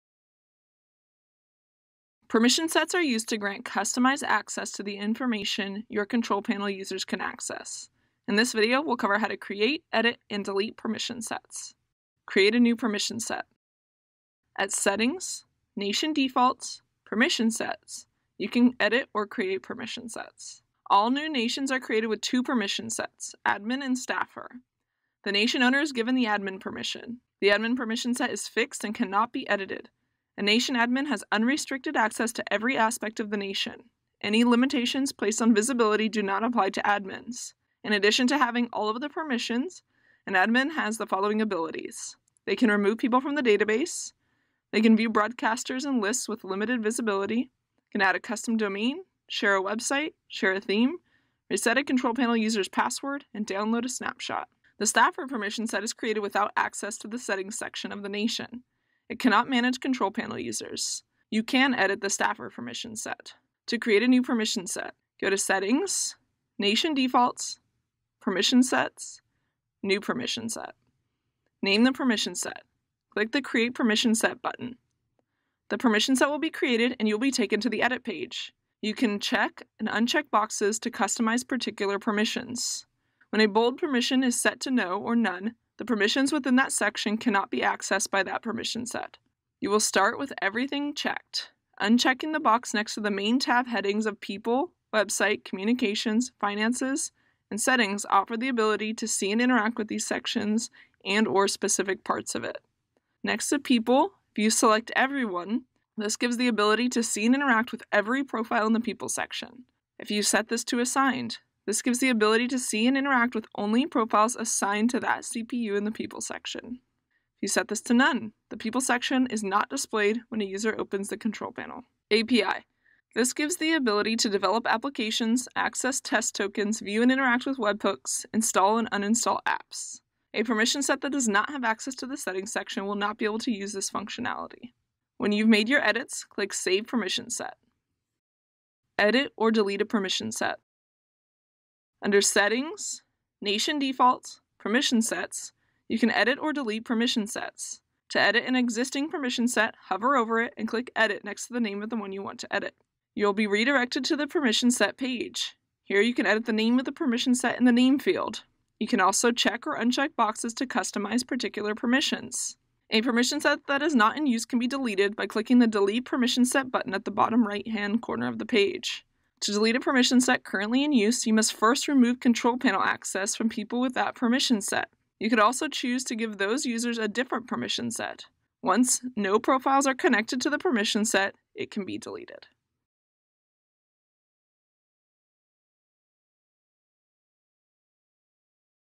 How to create, edit, and delete permission sets in NationBuilder
Permission sets are used to grant customized access to the information your control panel users can access. In this video, we’ll cover how to create, edit and delete permission sets.

👋Just a heads-up: This content was crafted back in 2019, so while it's packed with valuable insights, there may be newer developments worth exploring. Stay curious!